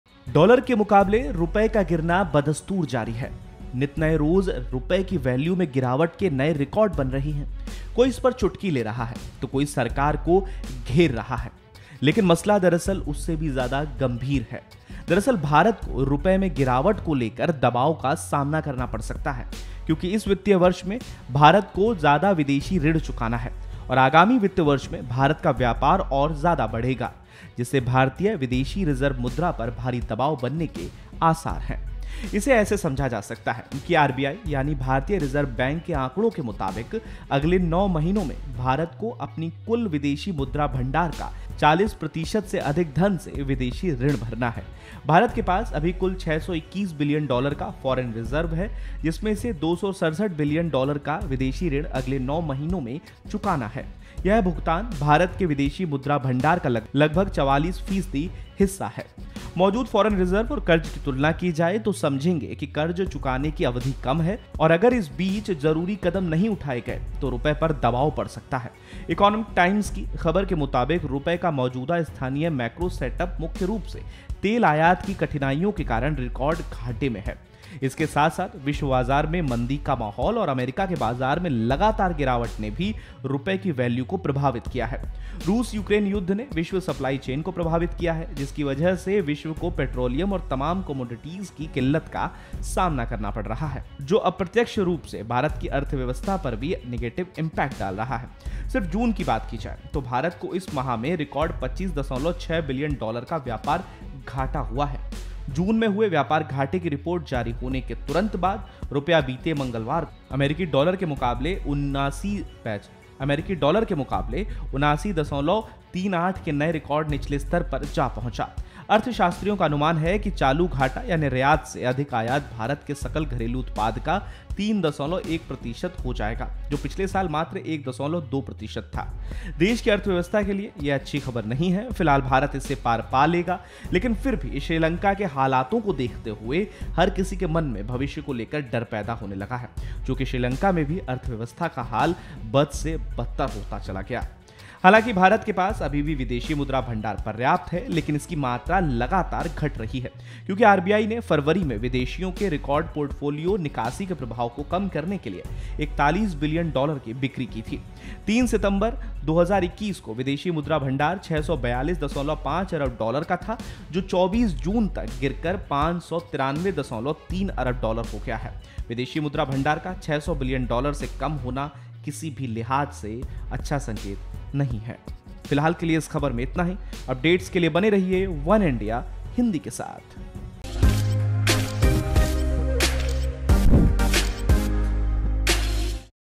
Alert: क्या श्रीलंका की रहा पर भारत ? रुपये में गिरावट, विदेशी कर्ज का बोझ | वनइंडिया हिंदी |*news
The fall of the rupee against the dollar continues unabated, new records are being made for the fall in the value of the rupee every day. Some are taking a jibe at it, some are besieging the government, but the issue is actually more serious than that.

डॉलर के मुकाबले रुपए का गिरना बदस्तूर जारी नित नए रोज रुपए की वैल्यू में गिरावट के नए रिकॉर्ड बन रहे हैं। कोई इसपर चुटकी ले रहा है, तो कोई सरकार को घेर रहा, लेकिन मसला दरअसल उससे भी ज्यादा गंभीर है।